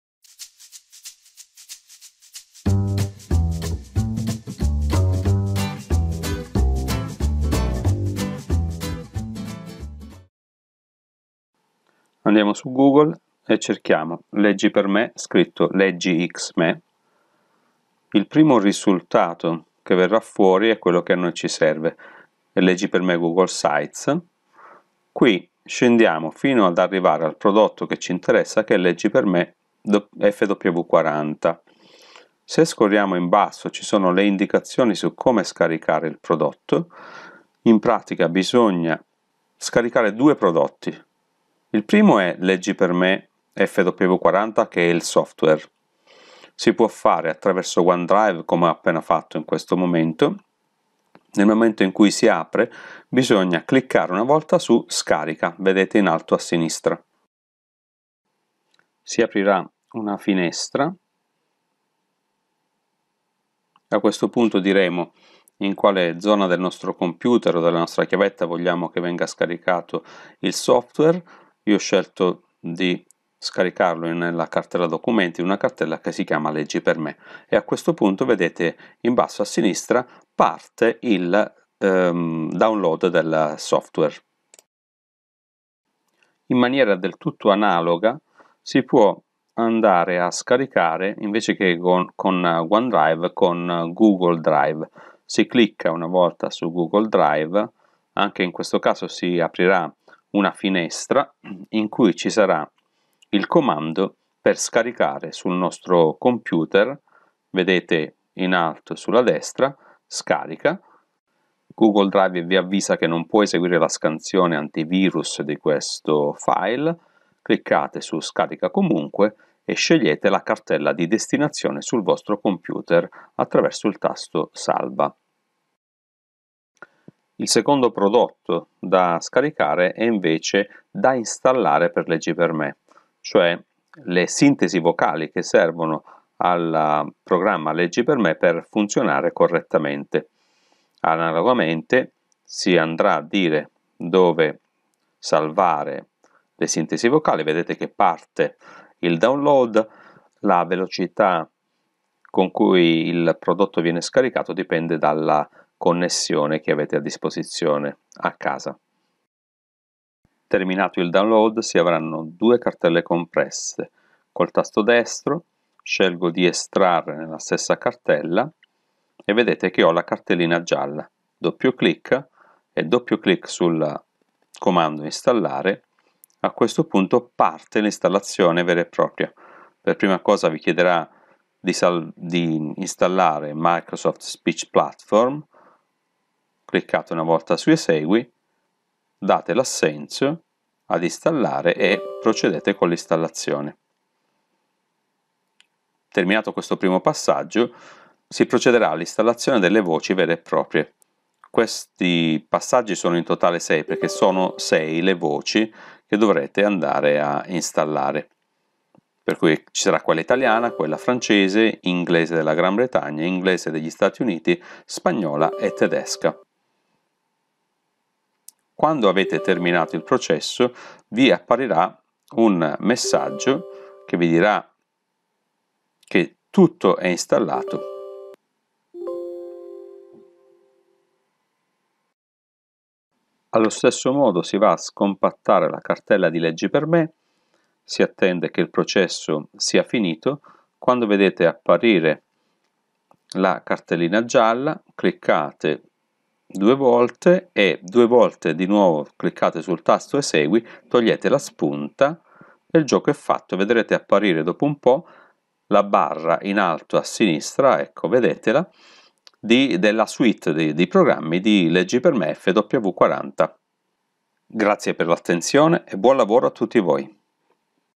Scaricare e installare LeggiXme FW40
Il video insegna come scaricare e installare il software gratuito LeggiXme.